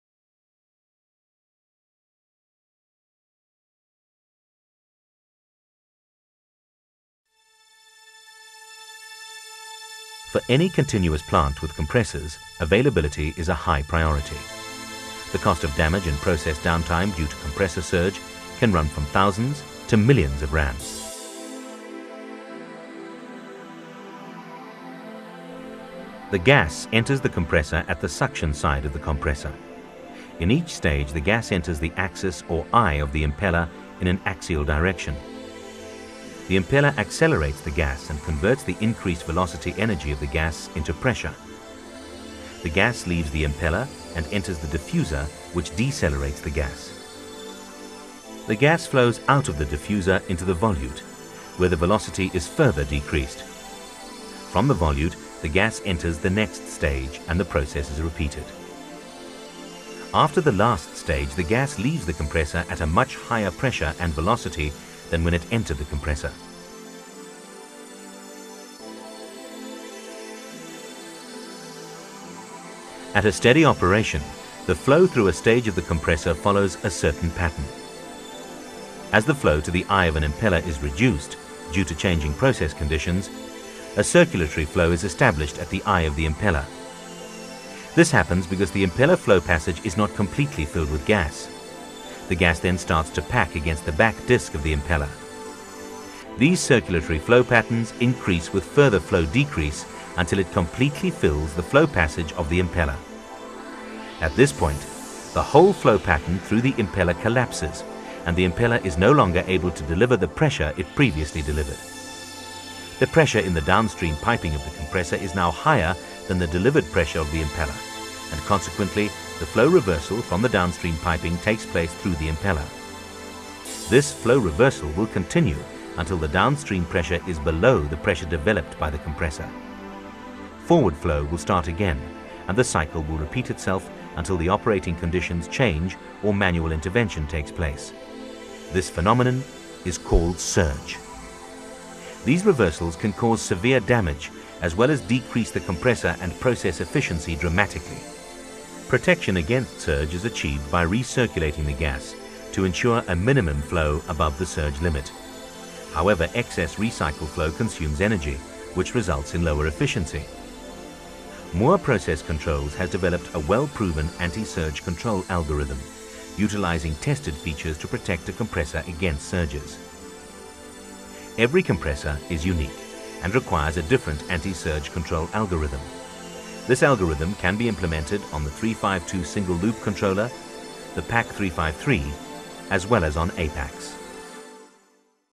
Compressor anti surge
Description of compressor surge, and methods to prevent surge from occurring